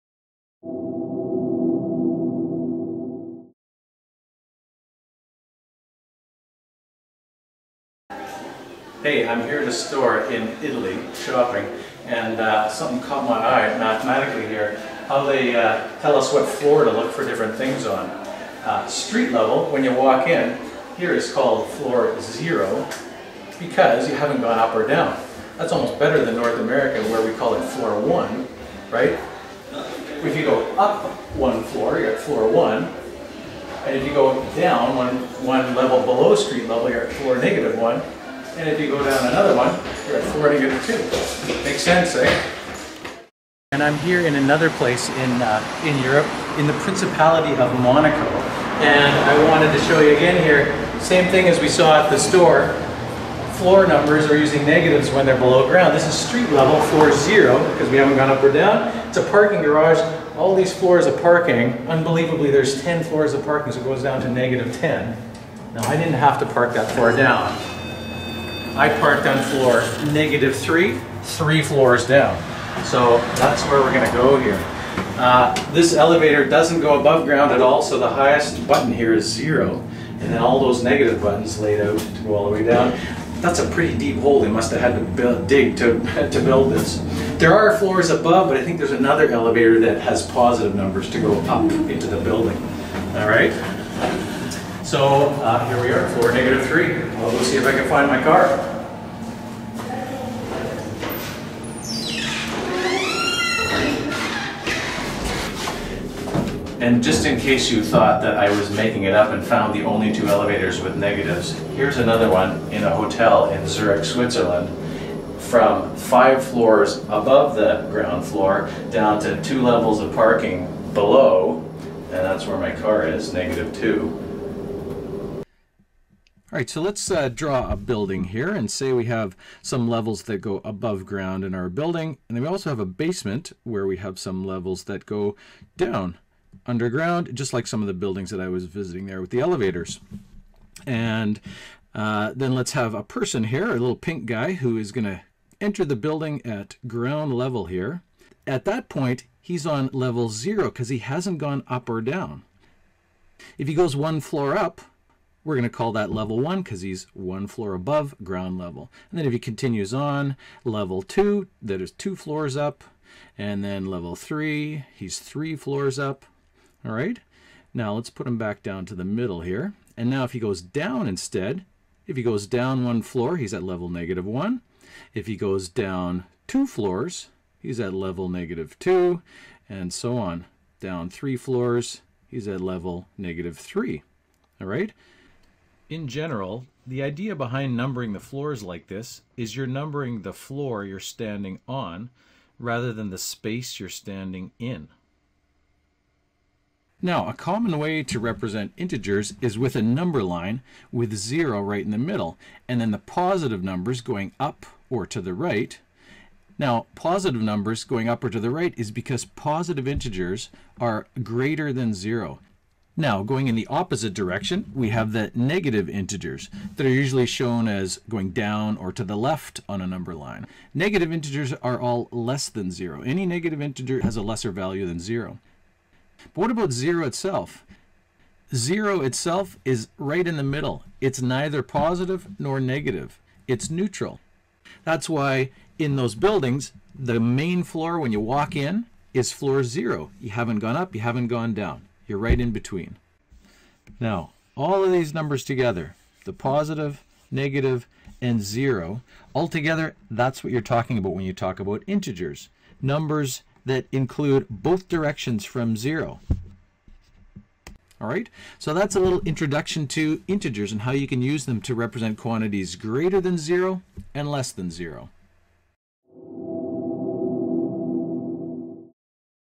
Integers... Floor Numbers in European Buildings [Real World Math!]
Looking for an example of integers in the real world? Here the concept of integer numbers highlighted in the floor numbering scheme used in European buildings.